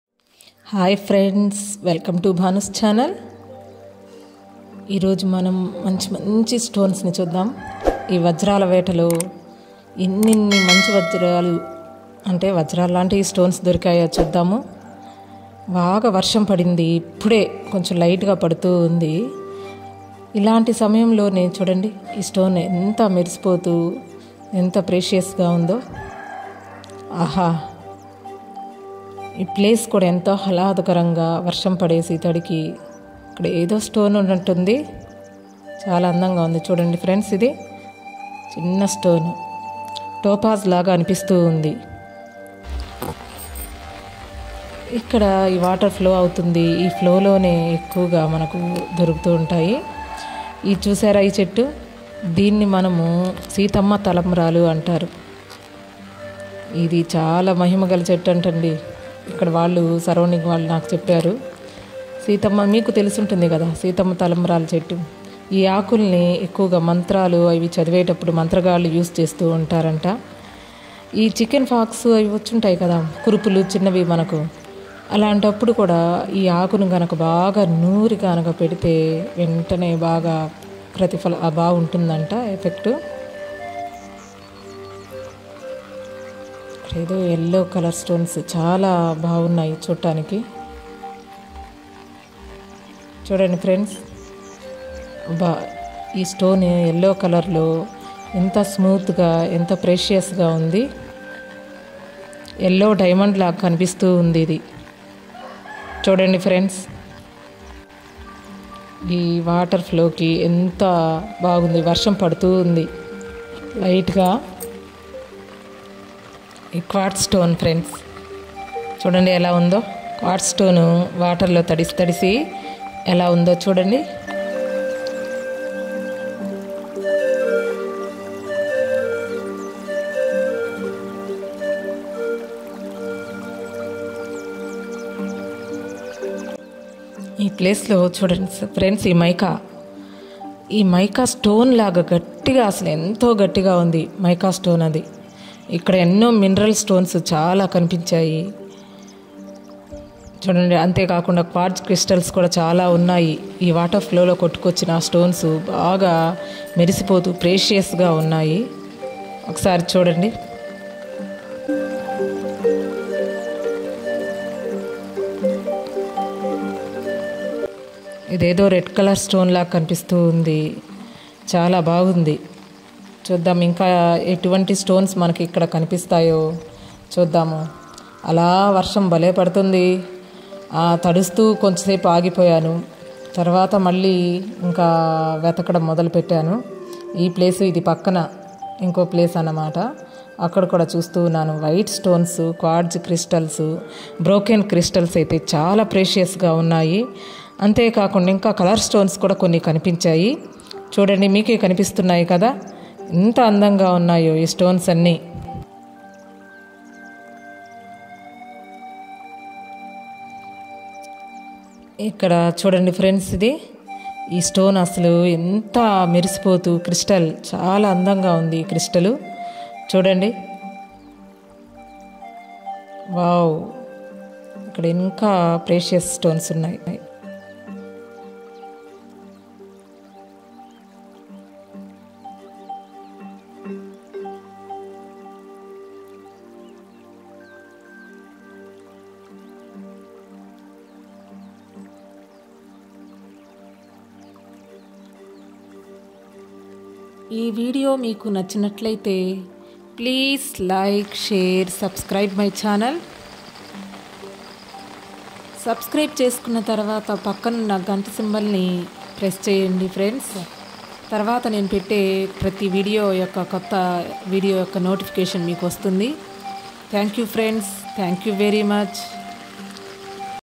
Diamond hunting in rain/ఎన్నో అందమైన Crystal stones కనిపించాయి/వర్షం లో వజ్రాలవేట/ bhanushaik vlogs
Hi friends 🙏
ఈ video లో ఎన్నో అందమైన క్రిస్టల్స్ వర్షంలో తడిసి మెరుస్తూ కనిపించాయి